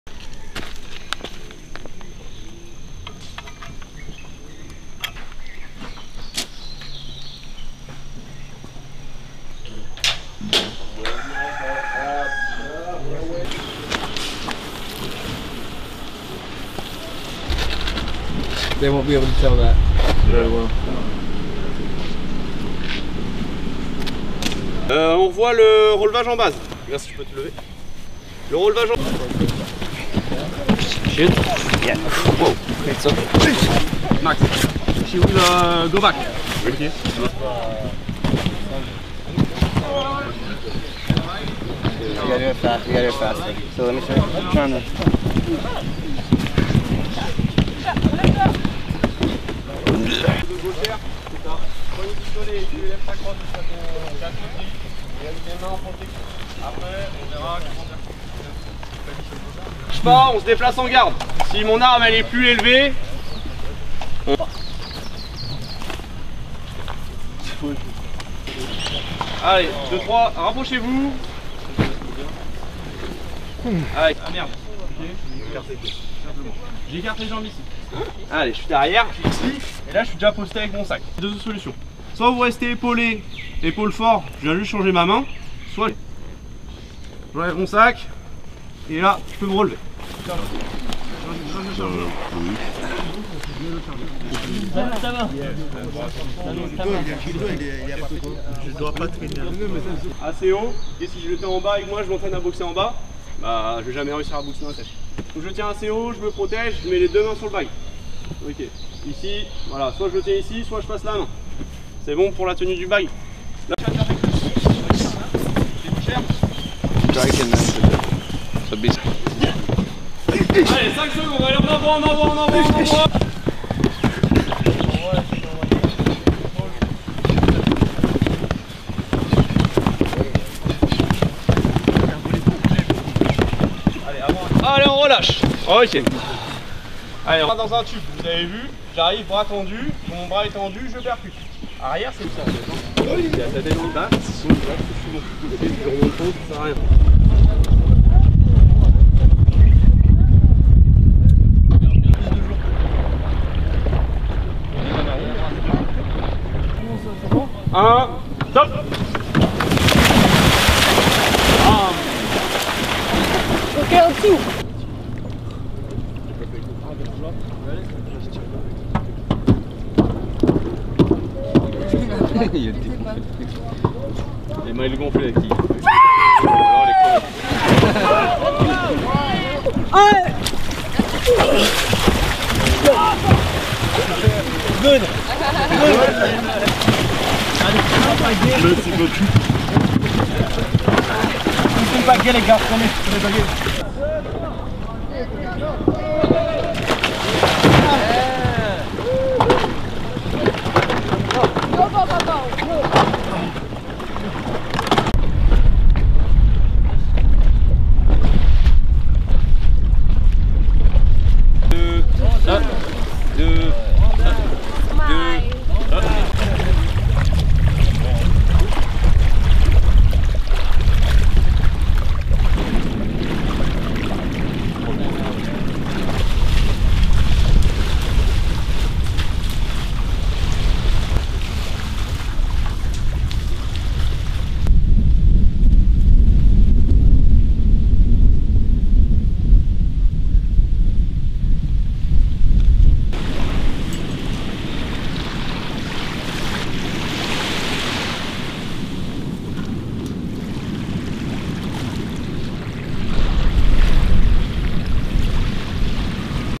MARFORPAC In Tahiti: Aito Course Combat Training and Beach Landings
U.S. Marines with Headquarters and Service and Battalion, U.S. Marine Corps Forces, Pacific, and members of the Marine Infantry Regiment in French Polynesia, French Armed Forces, conduct combat training and beach landings during the AITO Course in Taravao, Tahiti, French Polynesia, Sept. 7, 2022.

FRENCH POLYNESIA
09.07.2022

 The Marines participated in the bilateral training with the French Armed Forces for their AITO Course in order to strengthen relationships with the region. (U.S. Marine Corps photo by Cpl. Dillon Buck)

Film Credits: Video by Cpl. Dillon Buck
U.S. Marine Corps Forces, Pacific